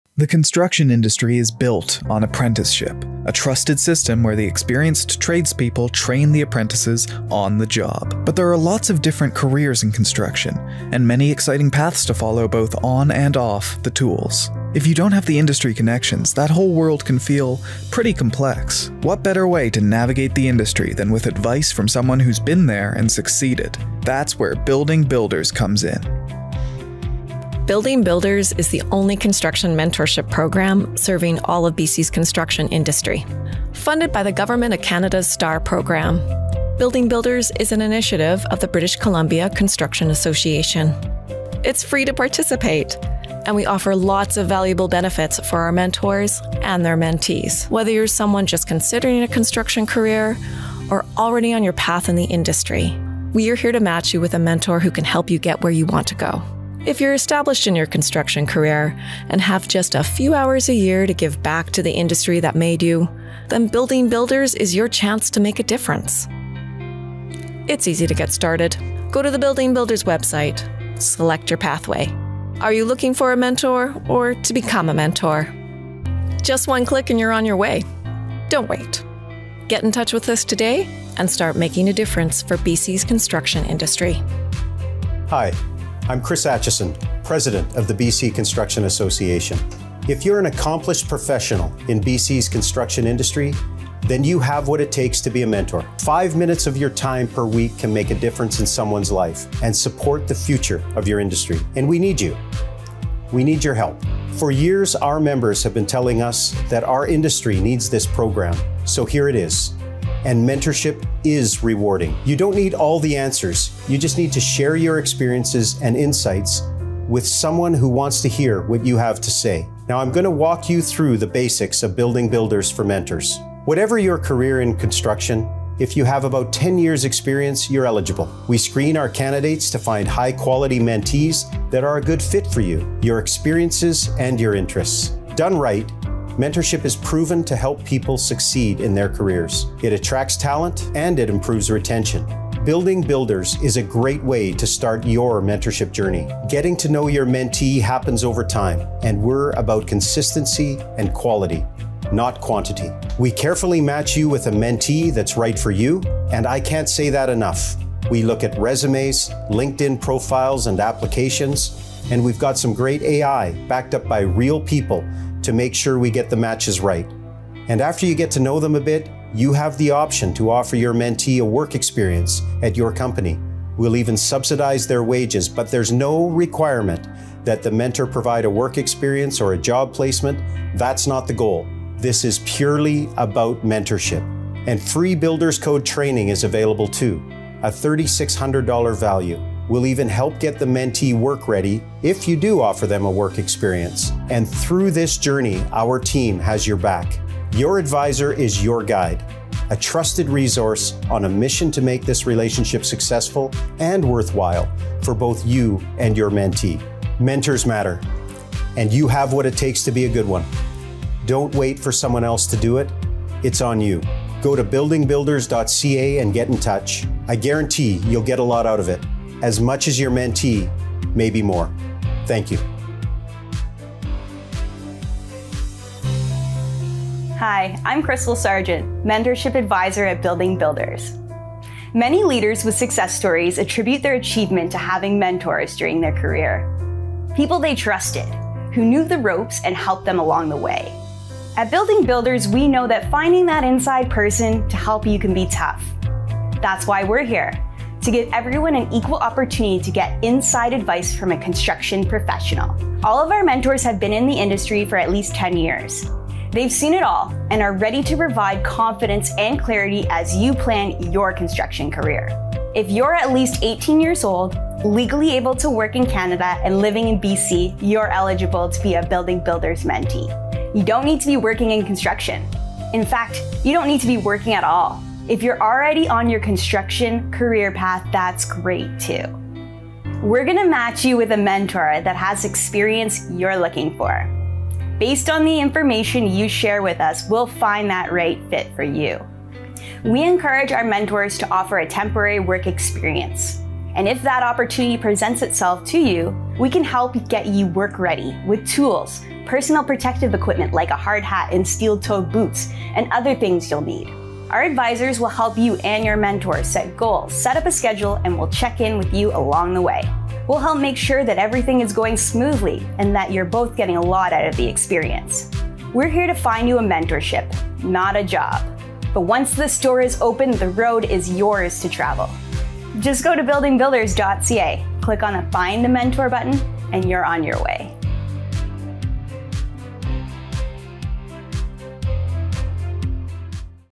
BCCA's Building Builders: A Custom Mentorship Program for BC’s Construction Industry
Connecting today’s experts with tomorrow’s leaders.

Mentors:
Today’s New Talent Needs You!

Building Builders is the Ideal Construction Mentorship Opportunity
It’s a minimal time commitment of only a few hours.
Your experience will be carefully matched to a mentee’s goals.
You receive free Builders Code training.
We can contribute to wages if you offer your mentee work experience.
We’ll be there to support you the whole time.

You don’t need special mentoring skills or experience to be an effective mentor. You don’t need all the answers. If you’ve been working in the construction industry for ten years and are willing to share your knowledge, you can make a real difference in a young tradesperson’s career.

Become a mentor today, and help shape the future of BC's construction industry.

Considering a career in construction?
Become a Mentee

Take Your Career to the Next Level
Mentorship is a Ticket to Success
The right mentor can help you build your career in construction by giving you advice on important things like which trade to pursue, how to talk to your supervisor, or when to make a change. They can connect you with opportunities and share insights that will make the industry easier to understand. You can meet in-person or virtually, and there’s no cost. The industry needs you!

Get real career advice from an experienced leader in the industry
Receive tools and equipment to start your career in construction
Get certified Health and Safety Training
Connect with someone who loves the industry and knows how rewarding it can be
We’ll support you and your mentor the whole time
Get paid work experience with construction employers

Who is Eligible?
You don’t need to have any construction experience, just a genuine interest in building a career in the industry.
If you meet the criteria below, let’s do this!
18+
A resident of British Columbia
Can commit to one hour per month to connect with a mentor
Want to grow personally and professionally
Have a desire to build a professional relationship with someone who will be in your corner

This program is administered by the British Columbia Construction Association.